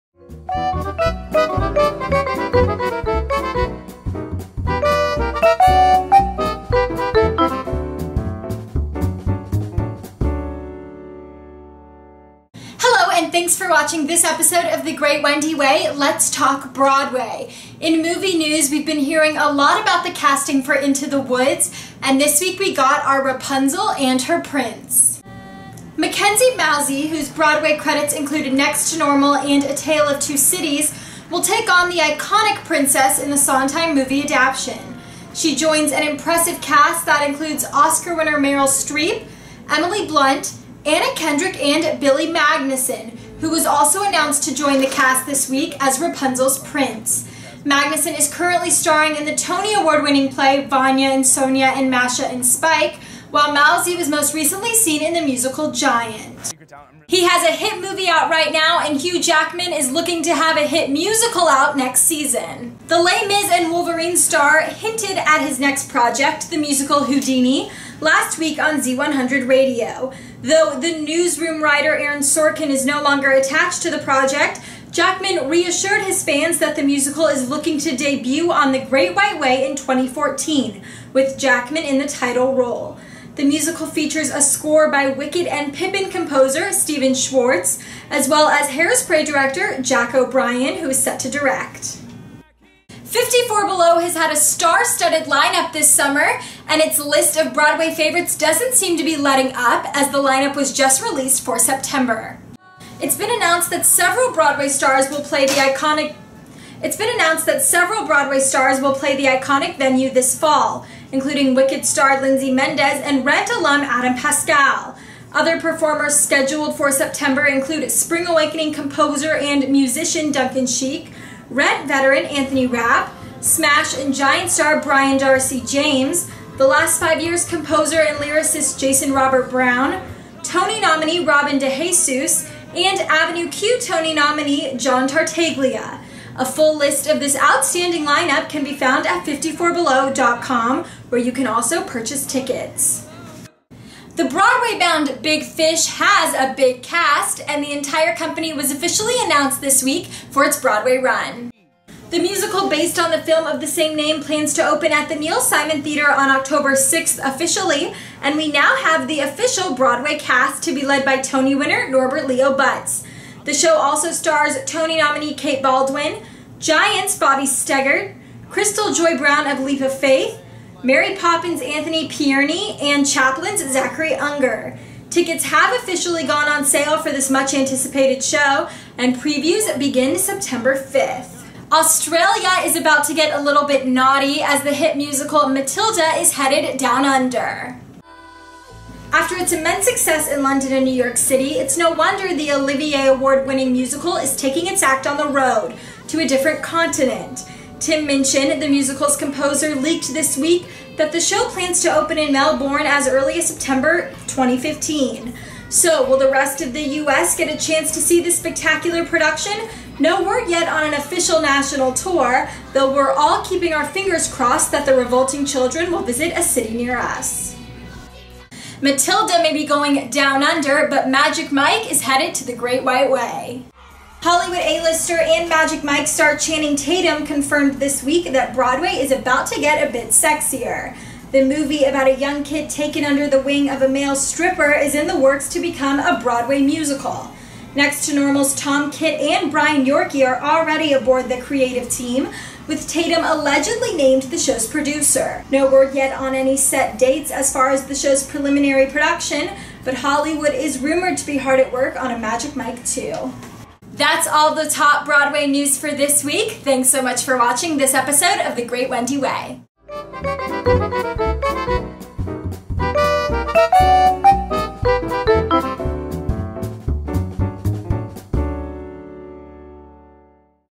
'The Great Wendi Way', Episode #41: Australia Gets 'Naughty' While Broadway Gets Sexy
In this episode of "The Great Wendi Way," StageDoorDish.com's Wendi Reichstein brings news of the upcoming 54 Below lineup, Matilda's move Down Under, Magic Mike headed to Broadway, and more Into the Woods casting. All this and more in this week's The Great Wendi Way.